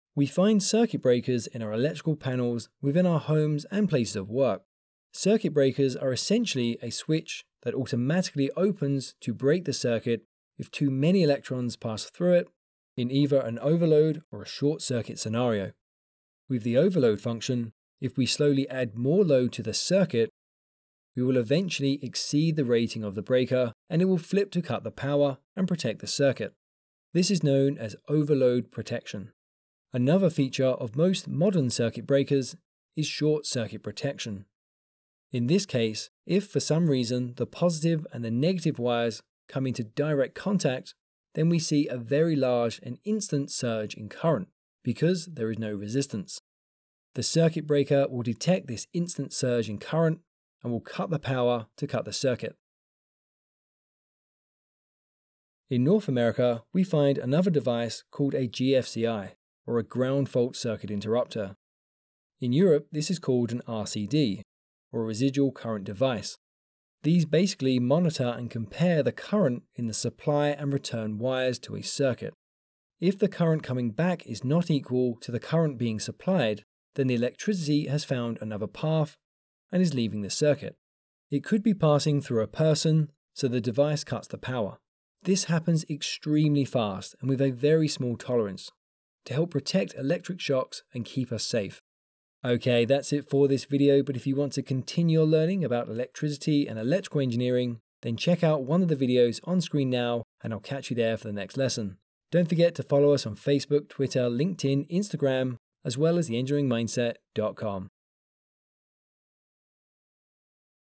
Circuit Breaker Basics - How do they work?
In this video, we'll discuss the basics of circuit breakers. We'll cover the types of circuit breakers and their uses, as well as the different types of circuit protection. After watching this video, you'll be able to understand and use circuit breakers effectively